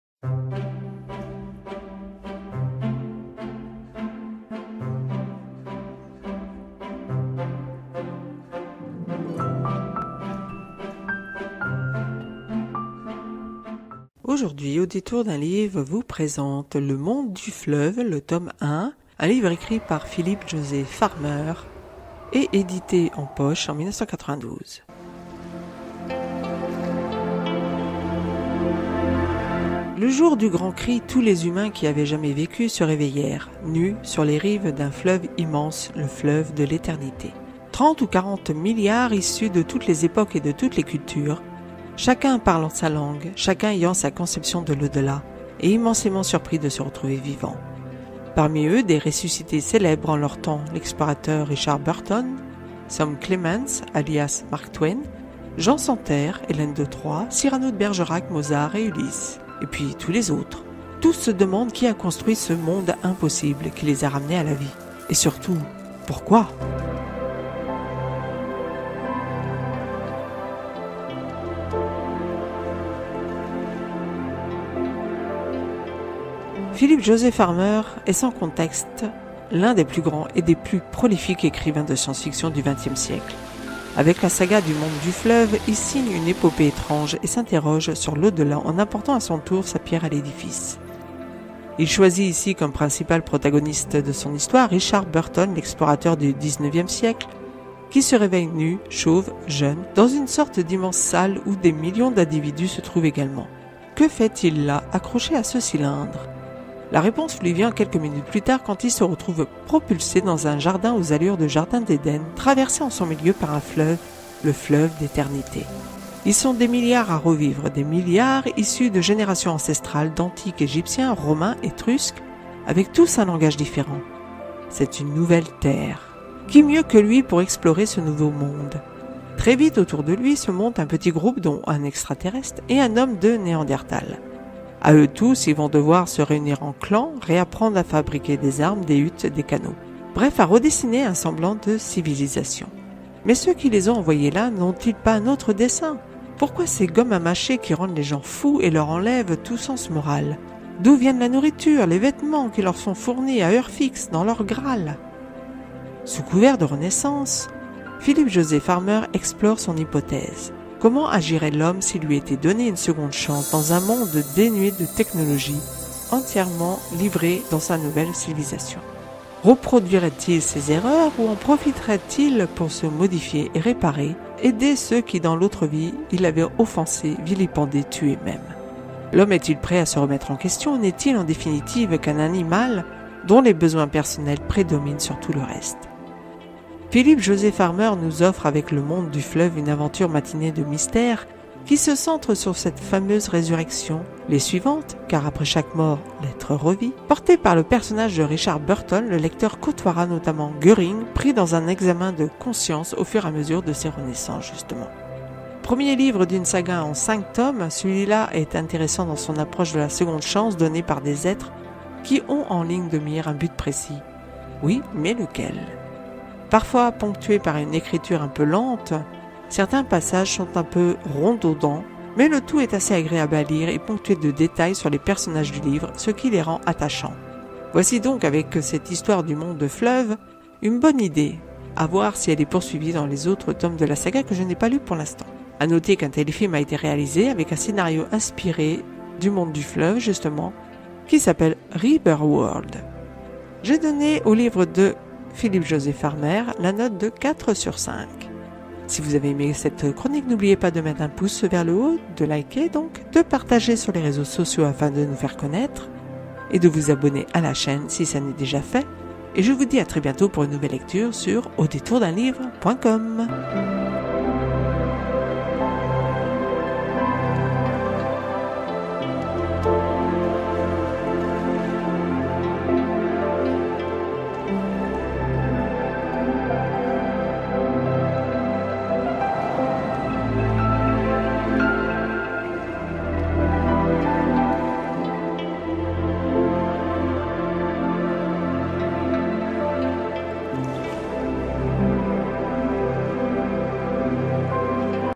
Le monde du fleuve, de Philip José Farmer (résumé et critique)
Avis et revue du livre de science-fiction, le tome 1 écrit par Jose Philip Farmer "Le monde du fleuve"